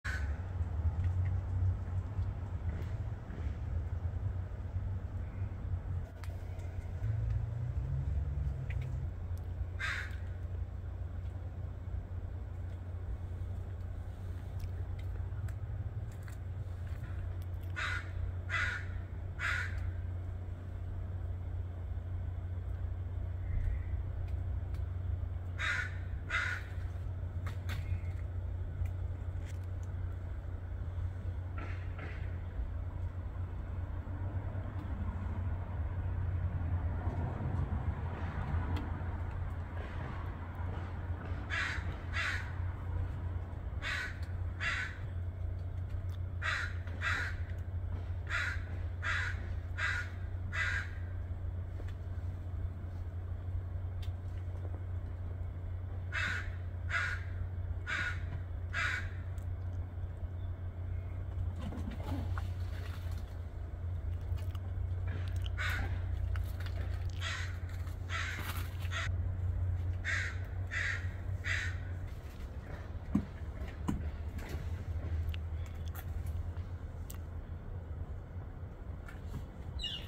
the morning crew
The morning crew! Sorry for the horrible mobile phone video, they move too much.
It is nice to have people always wait for you in the morning, and I have the whole crew. As soon as I open the house door, jays always show up first. Very impatient. They start banging their beaks on the branches, making sounds similar to the empty spoon banging a pot: we want our breakfast, and we want it now, hurry up man!
Next a line of gray squirrels is already forming on the fence. One, two, .., eight. Fluffy butts need to eat too.
The crows outnumber everyone else, but are most shy. They always wait until I go back inside to start eating.